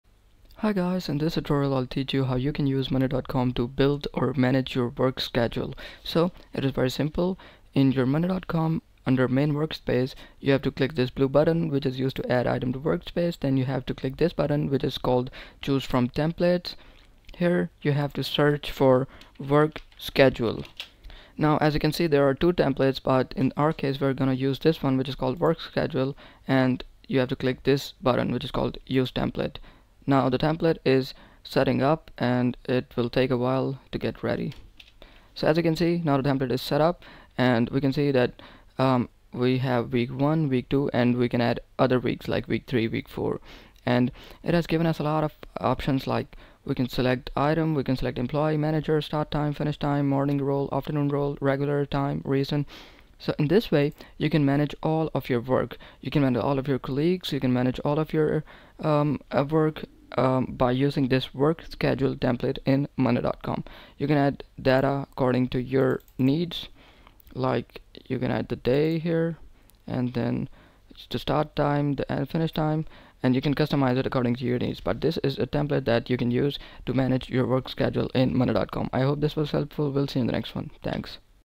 How to Use Monday.com to Build Work Schedule [Quick Guide]
In this tutorial, we will show you how to build work schedule on monday.com in a few simple steps!